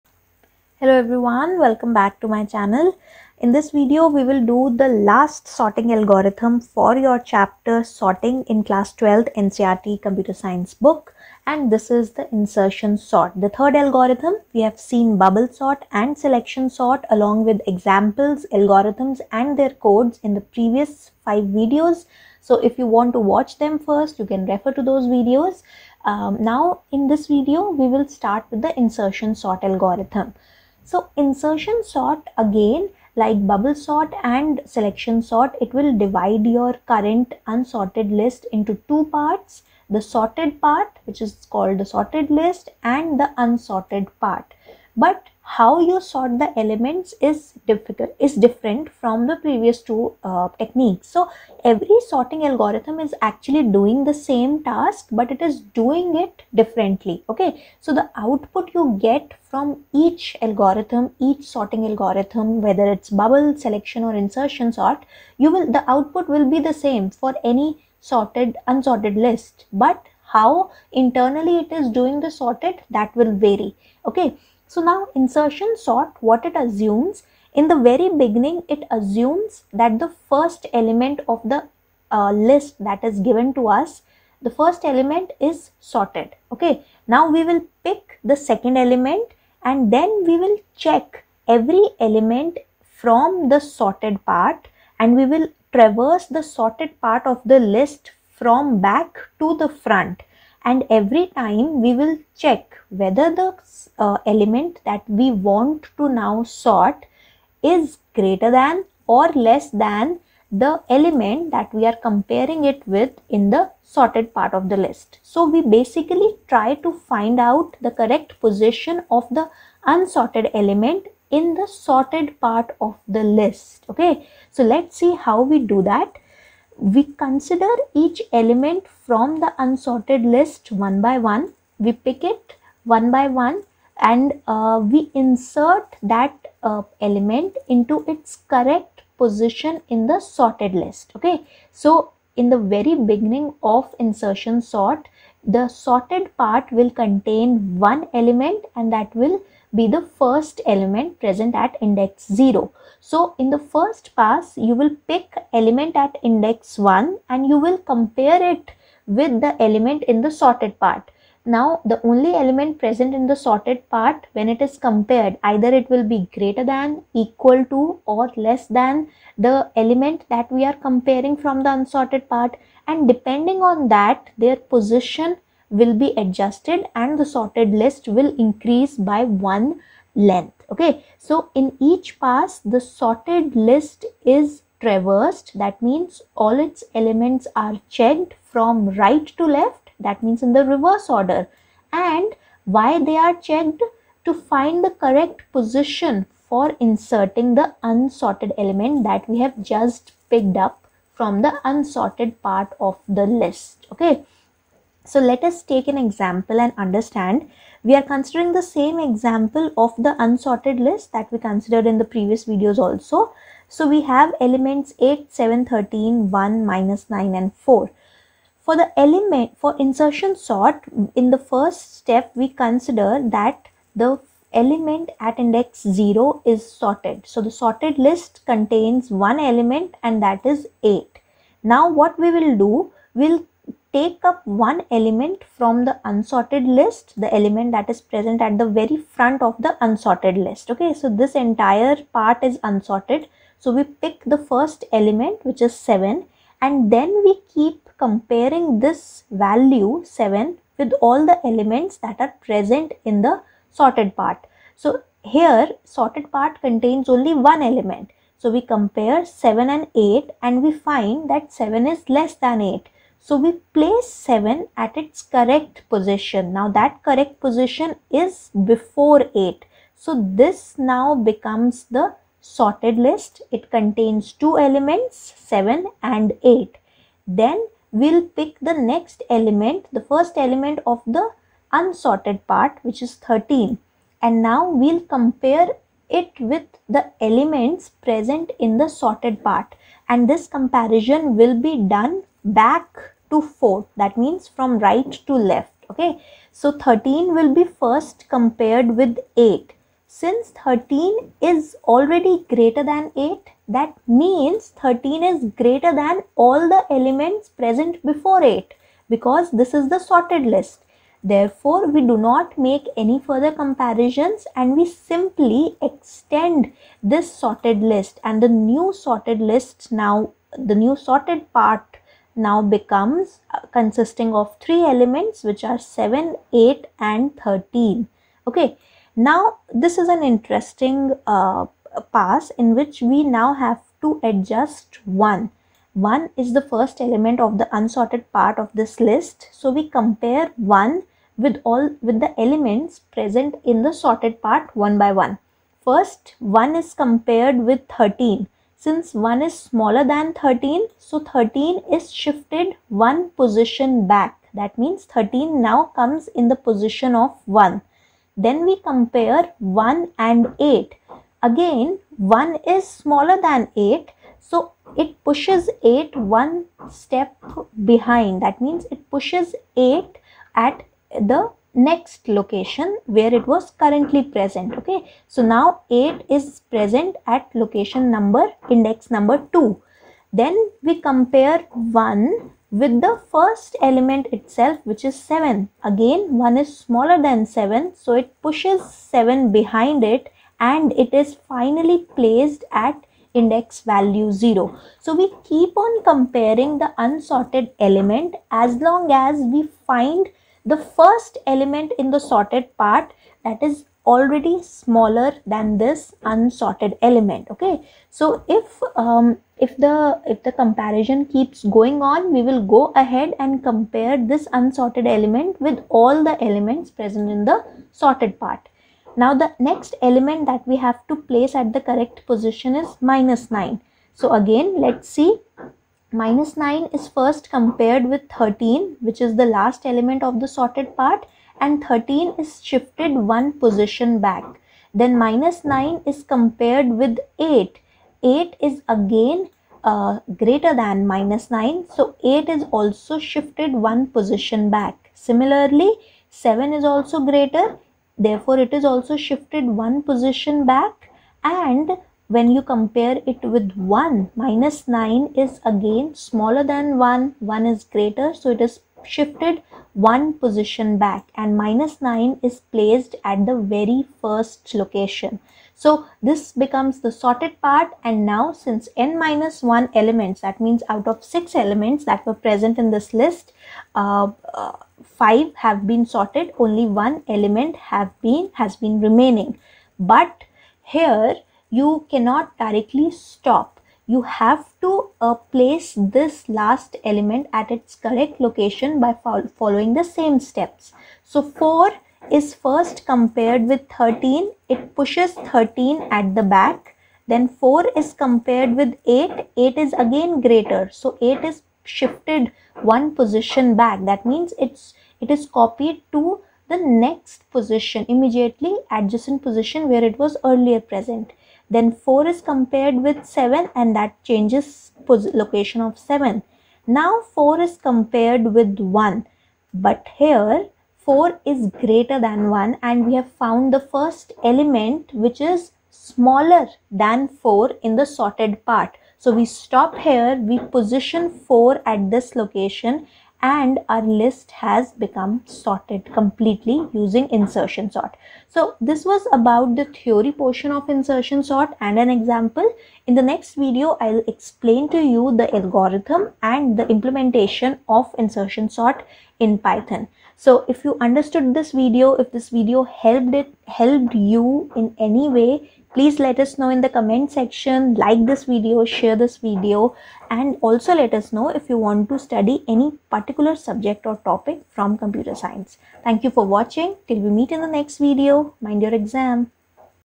Insertion Sort Explained with Example | NCERT Class 12 Computer Science | CBSE Class 12 Board Exams
NCERT Class 12 Computer Science with Python 3
CBSE Class 12 Board Exam
Chapter 5: Sorting

This video explains the Insertion Sort algorithm with an example.